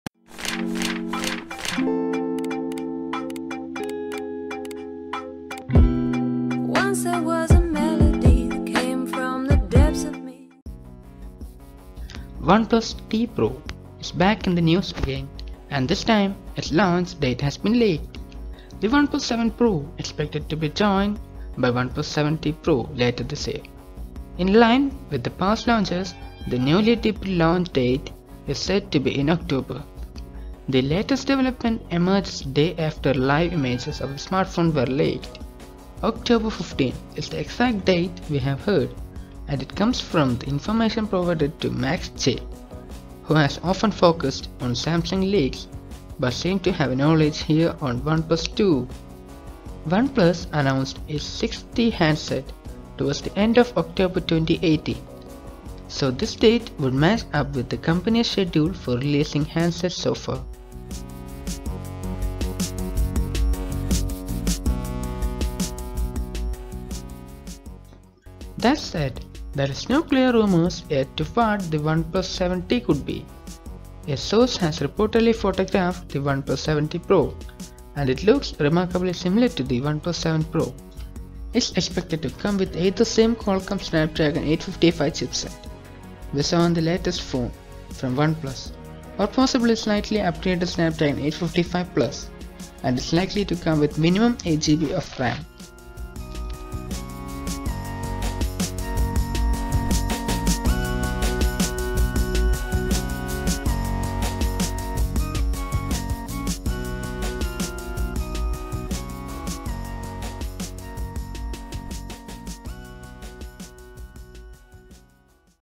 OnePlus 7T Pro: Launch Confirmed!!!
In this video you will be getting all details about OnePlus 7T Pro: Launch Confirmed!!!
As OnePlus 7T Pro: Launch Confirmed, The information surrounding the upcoming OnePlus smartphones come from Max. J, who is known for primarily leaking information on samsung, as evident from his username, @Samsung_News_. Also, no other information surrounding OnePlus 7T series has been leaked by Max. J, who only tweeted a 1+, with an image saying October 15.

Comments will be read and please post feedback on videos.